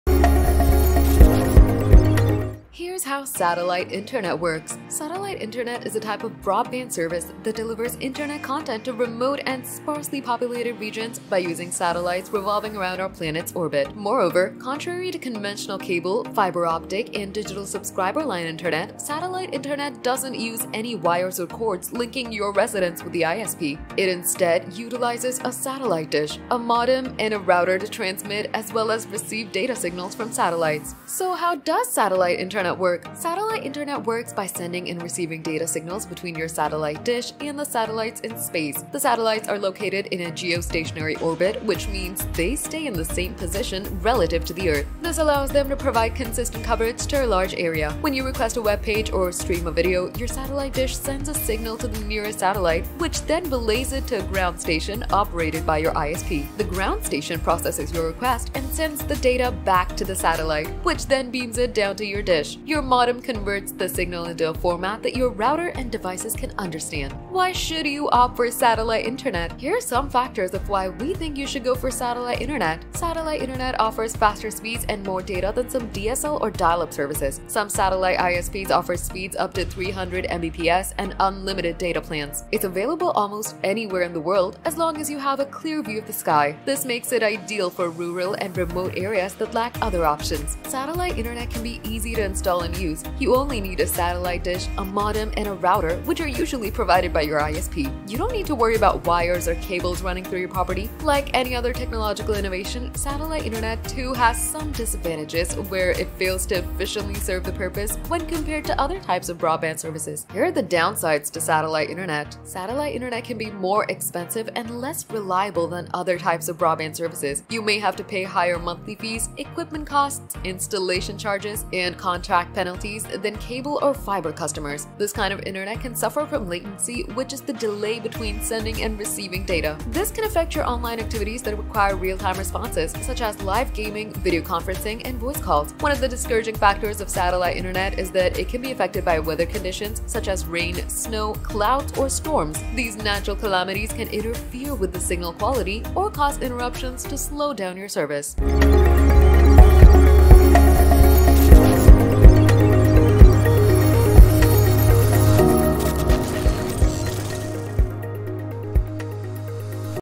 Satellite internet: How does it work | Gad Insider
Satellite internet is a type of broadband service that delivers internet content to remote and sparsely populated regions by using satellites revolving around our planet's orbit.

Moreover, contrary to conventional cable, fibre-optic, and Digital Subscriber Line (DSL) Internet, satellite internet does not use any wires or cords linking your residence with the ISP.